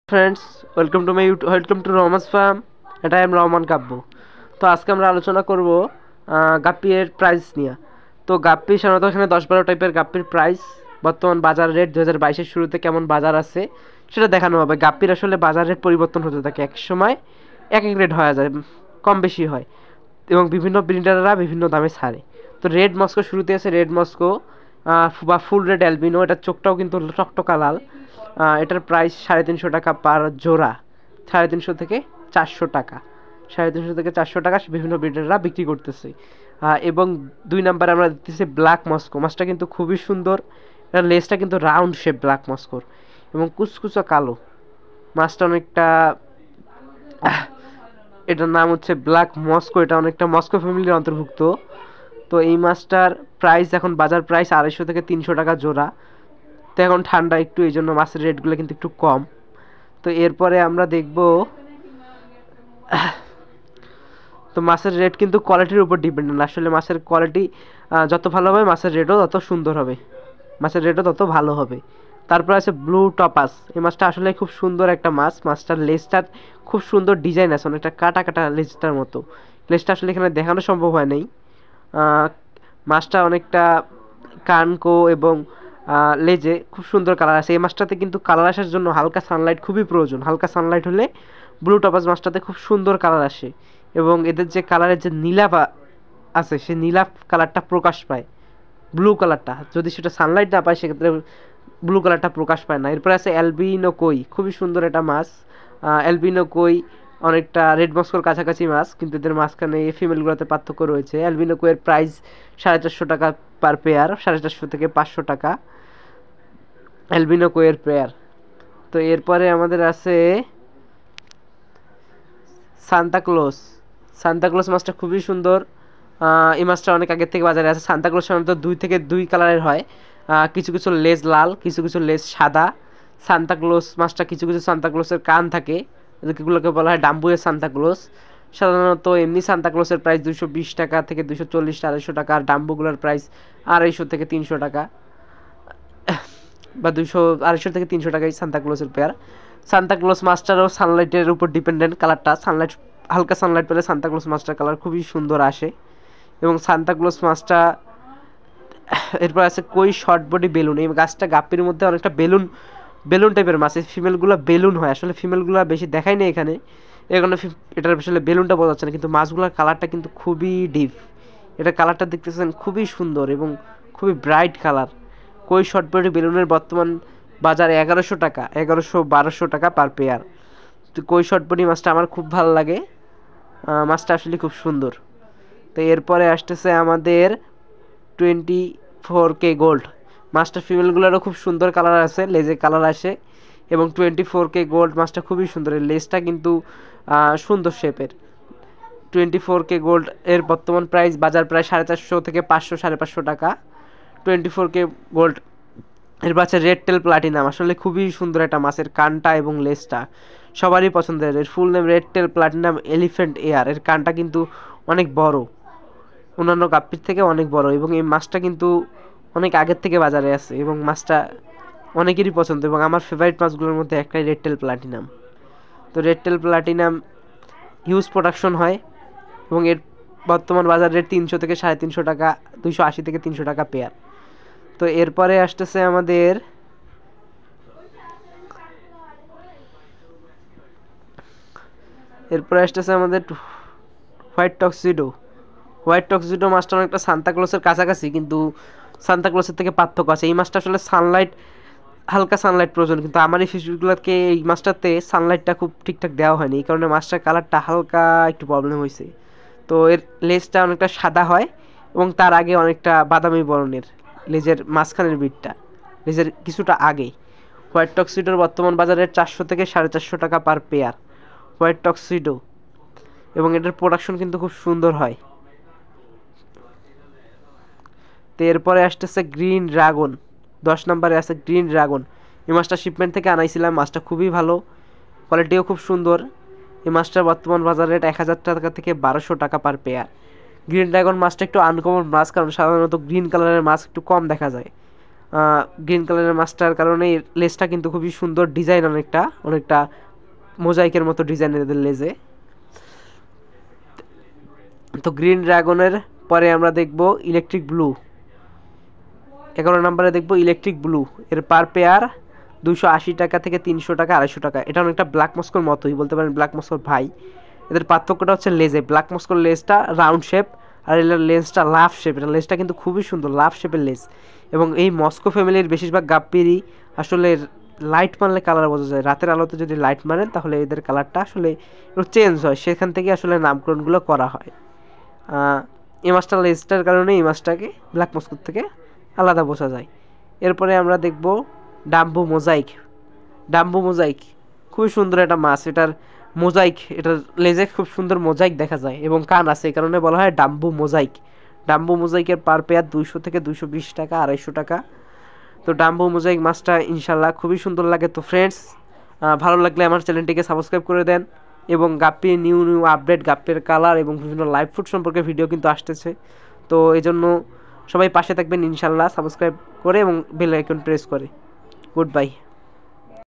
Top 12 fancy gappi price Bangladesh 2022
১২ রকমের গাপ্পির পরিচয় ও‌ দাম ২০২২.Wellcome to Rahman's Farm
fancy gappi price in 2022.This vedio will show update fancy gappi price 2022.New gappi collections and those update price.
beautiful gappies. we will talk you about 10-12 top beautiful gappi in bangladesh and those price. In that vedio you will know many kinds of gapopi fish name and those price.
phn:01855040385